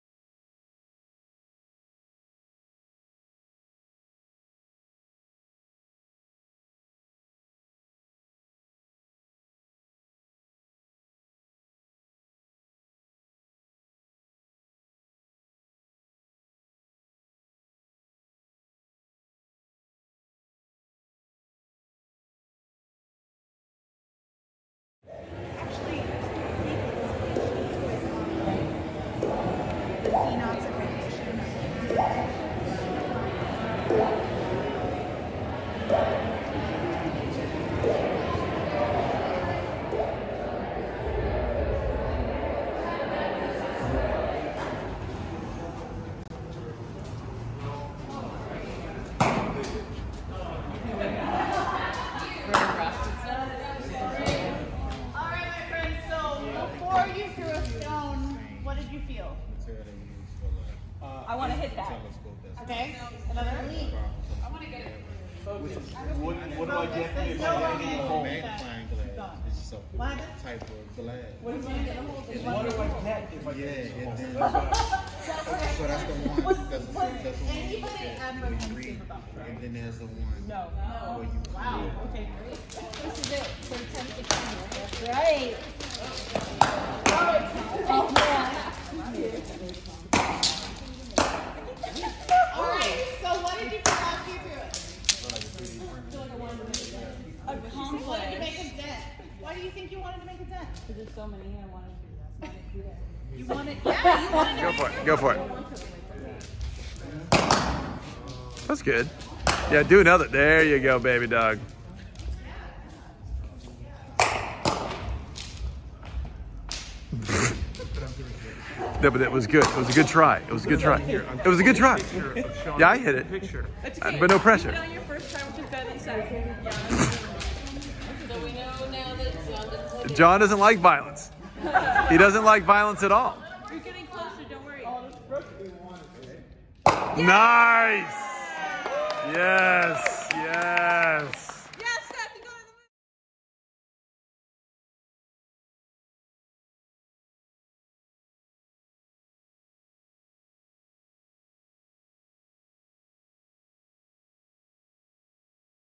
fail
We attended the opening night of fail at the Contemporary Art Center in New Orleans the evening of March 12, 2022.

fail was a multimedia group exhibition that brought together works by twenty five artists exploring systemic failures facing our world.

Examples of the works highlighted in our video:

You Used Me Lover Earth (2016). Multimedia (green screen segments)
Agustina Woodgate's Work Out (2019). Installation (Synchronous analog clocks)

The majority of our video featured students interacting with:
Sinisa Kukec's Sympathy for the Stone (2022) exploring violence with stone-throwing at a model of the Webb Space Telescope mirror array.